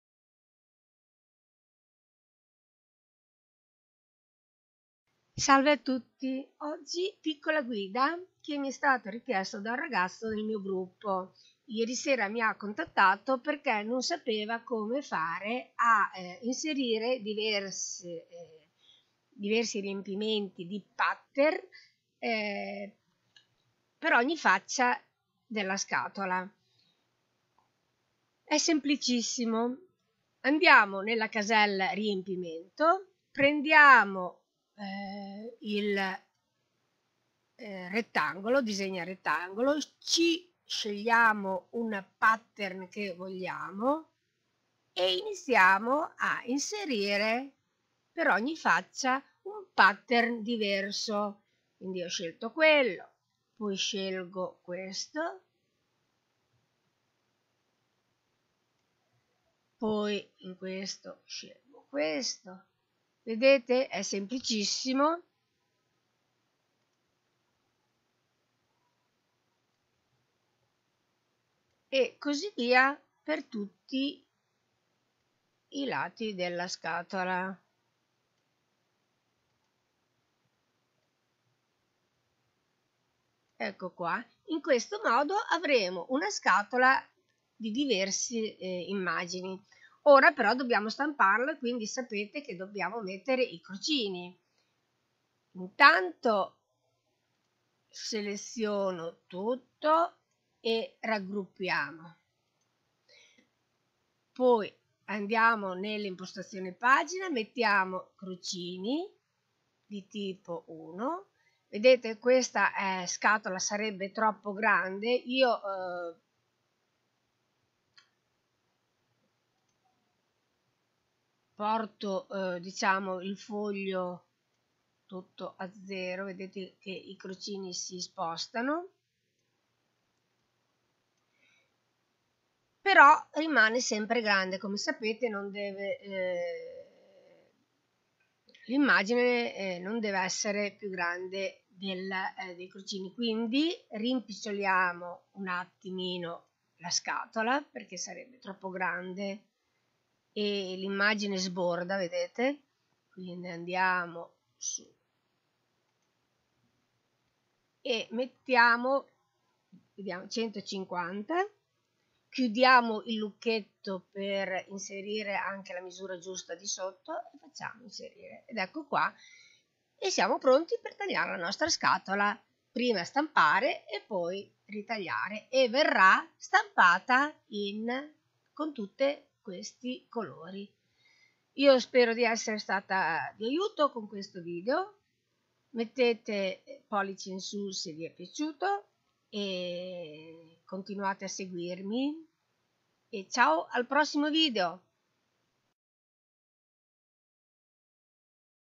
piccola guida in italiano :come inserire diversi pattern nella scatola
Piccola guida richiesta da un ragazzo ,come inserire diversi pattern in ogni faccia della scatola e  stampare

Eseguo lavori di scrap,e timbri personalizzati, accetto anche su  richieste, per qualsiasi informazione sui lavori, anche  per info su  prodotti Silhouette